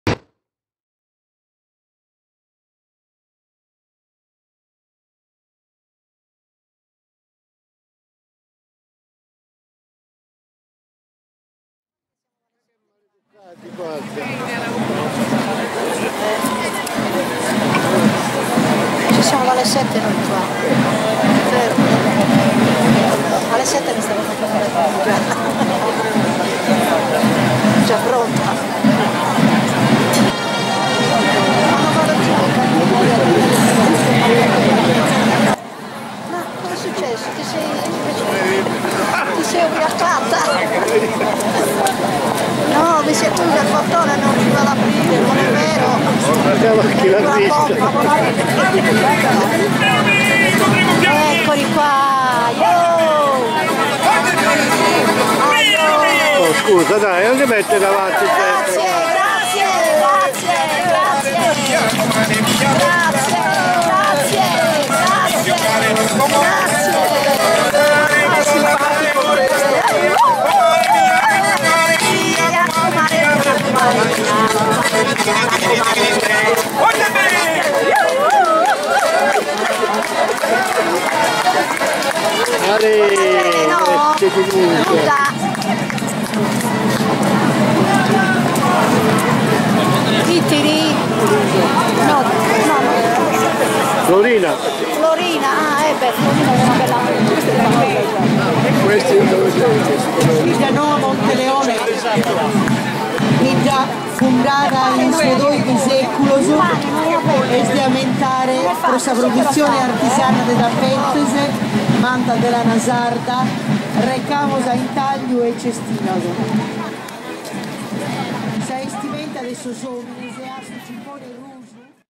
Sassari: Cavalcata Sarda 2019 - Florinas
Sassari, Cavalcata sarda 2019. Breve video dedicato alla sfilata del gruppo folk Figulinas di Florinas.